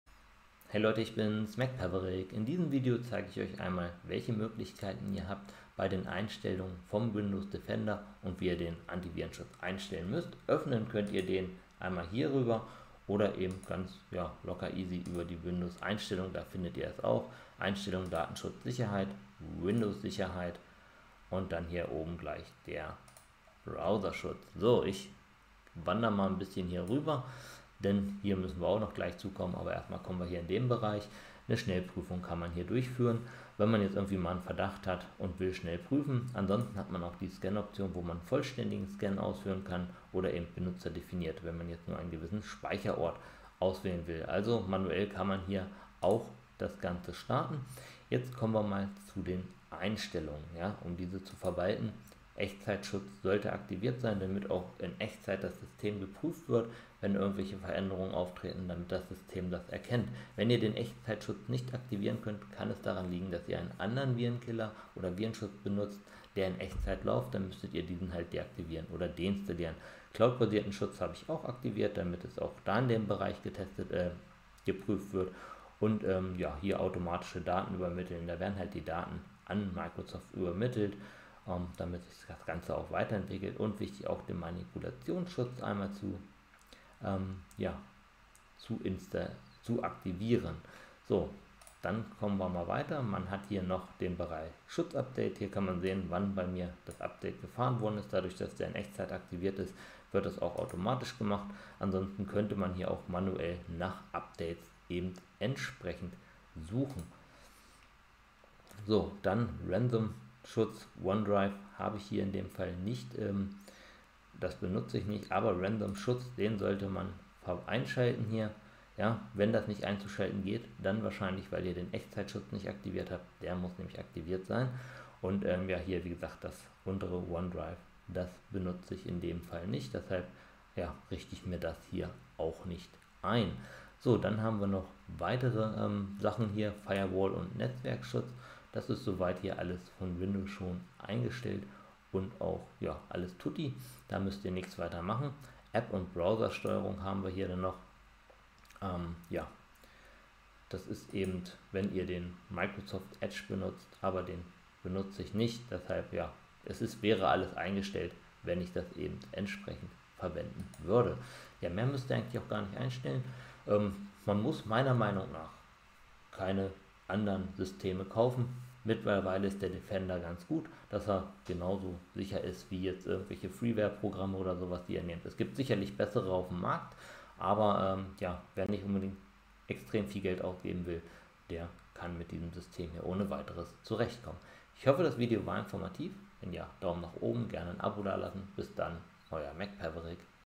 Windows Defender richtig einstellen & nutzen + Ist der Windows Defender ausreichend?
Windows Defender Sicherheitscenter Einstellungen - YouTube Video Anleitung Ich zeig euch, wie Ihr den Windows Defender aktivieren, deaktivieren und richtig einstellen könnt unter Windows 11